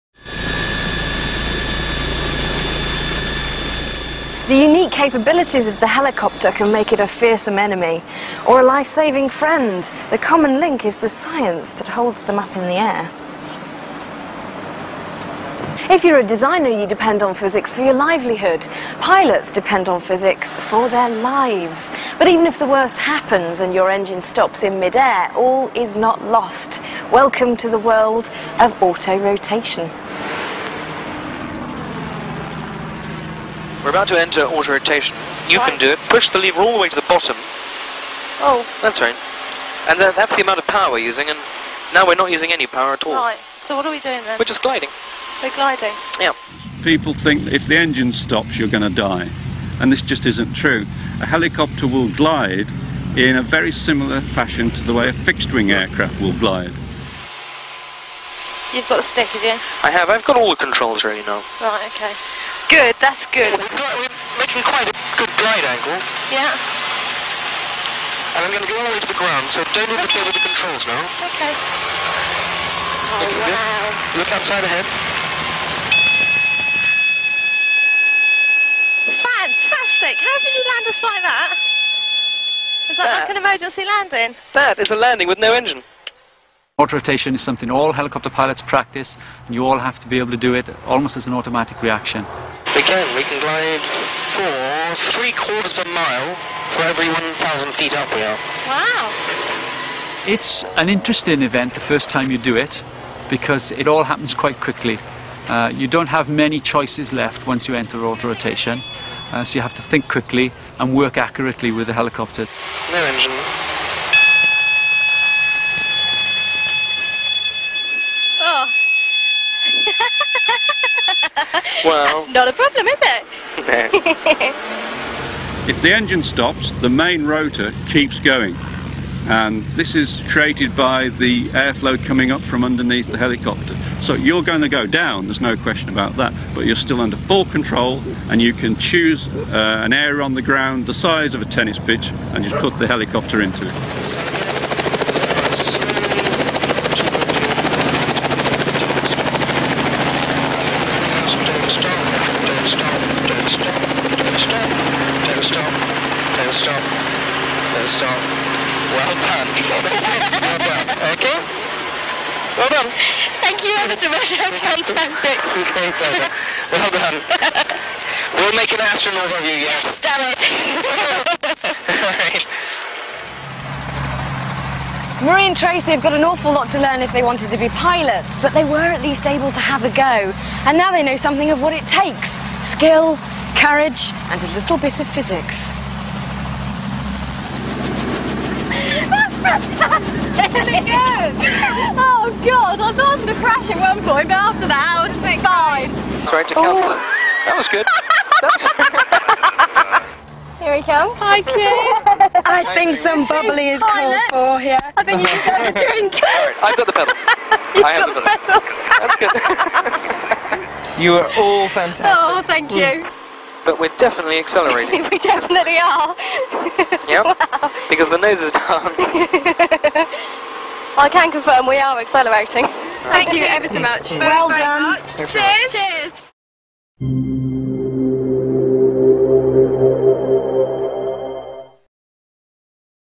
Autorotation's With Q - HQ Aviation Ltd. (Part 2/2)
Featuring footage from the BBC's Open University, Q Smith teaches budding helicopter pilots the principles of autorotation.